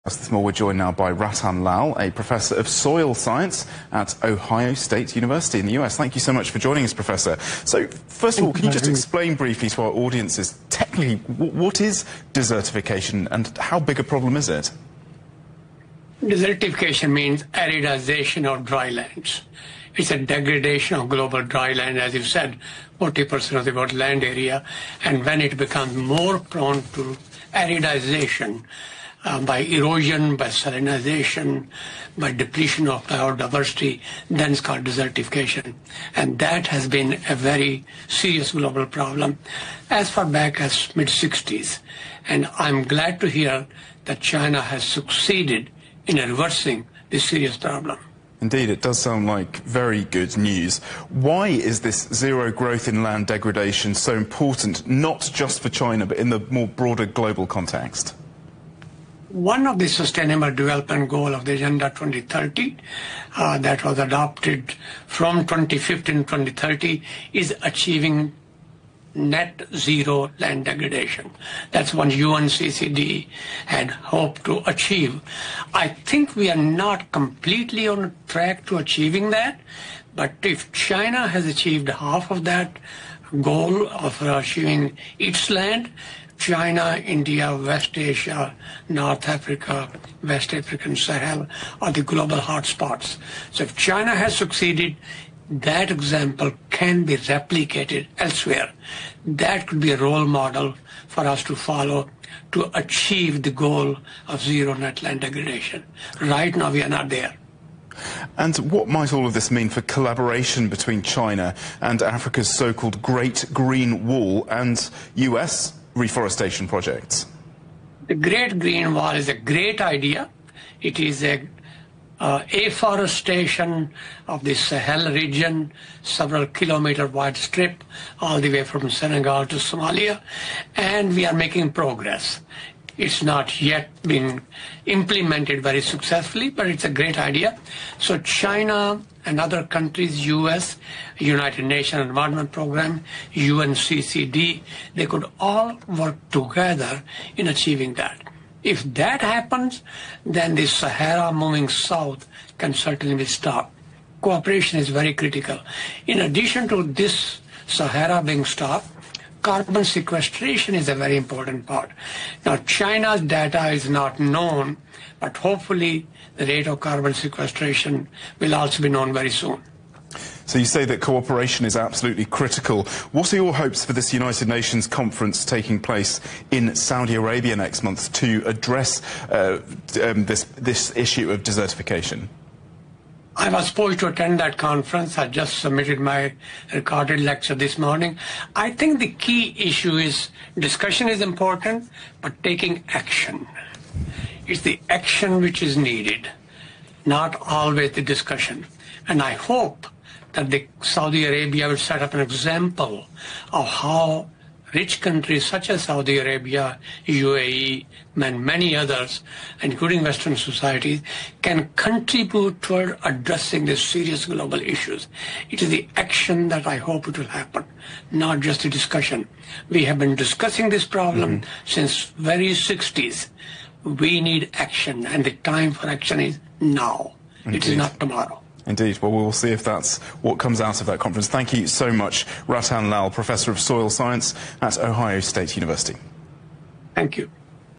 How China is Leading the Fight Against Desertification?
Rattan Lal, Distinguished University Professor of Soil Science at the Ohio State University spoke to CGTN Europe about China’s achievements on combating desertification.